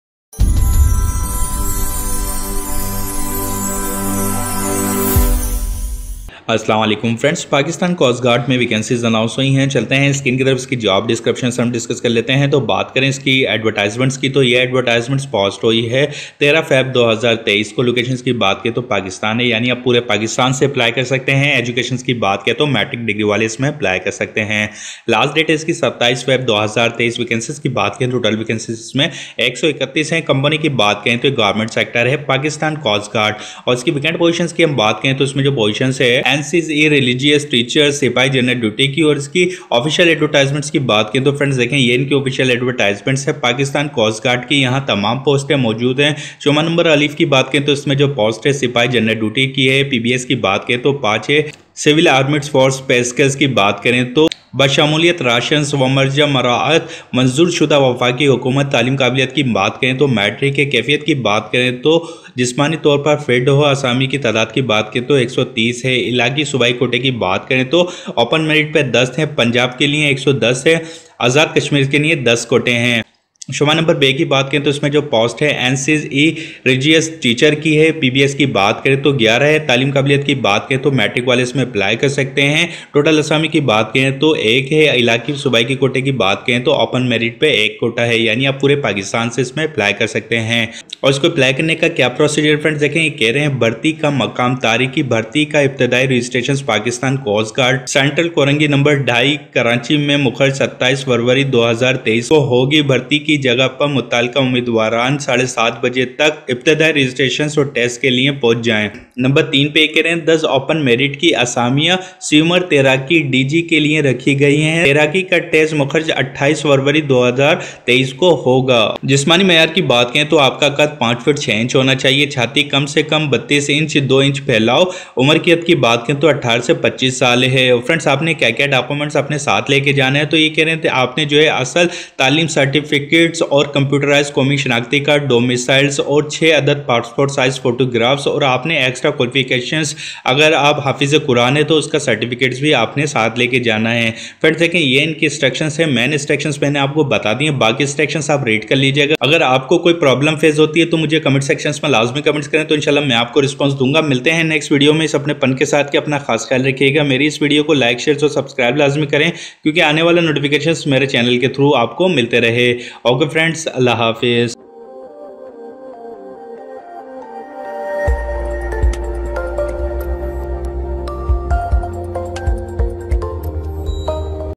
Pakistan Coast Guard Solider jobs | Online PGC Registration | Latest Pakistan Coast Guard Jobs 2023
Pakistan Coast Guard Solider jobs | Online PGC Registration | Latest Pakistan Coast Guard Jobs
((((( Advertisement LInk & How to Registratio ?)))))

Vacant Positions:
N Seas (E) Religious Teacher
Sipahi General Duty

Offline/Online PCG Registrations:
Interested persons can directly appear at Pakistan Coast Guards Center, Korangi No. 2, Karachi, on 27th February 2023 for Registration.
Candidates must bring original documents with attested copies of these documents, including academic certificates, domicile, CNIC, and recent photographs.
To get recruited by Pakistan Coast Guards, candidates must go through an Initial Test, Physical Test, Written Test, Medical, Interviews, Final Selection, and Appointment Letter.